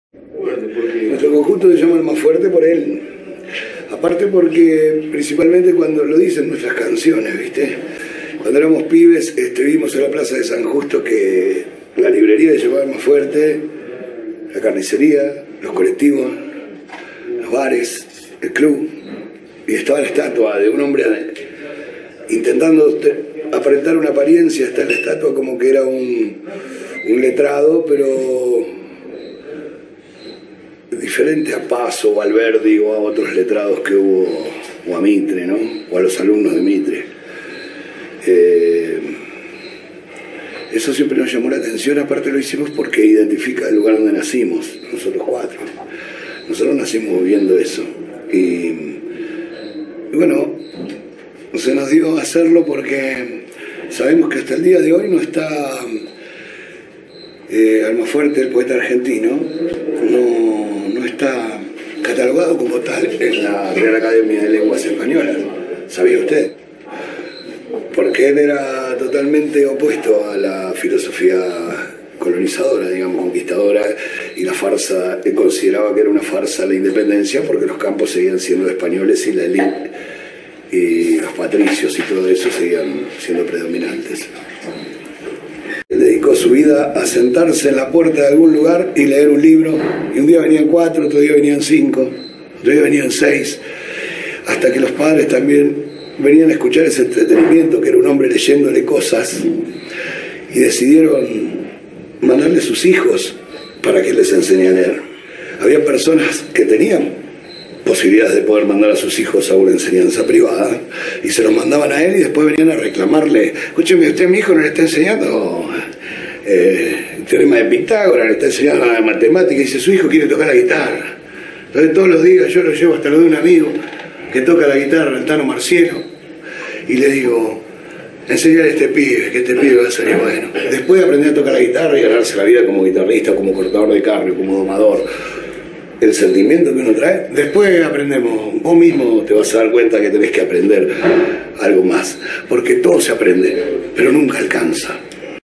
Iorio hablando sobre el maestro y poeta Almafuerte
Pedro Bonifacio Palacios, conocido también por el seudónimo de Almafuerte, fue un maestro y poeta argentino, considerado como uno de los «cinco sabios» de la ciudad de La Plata.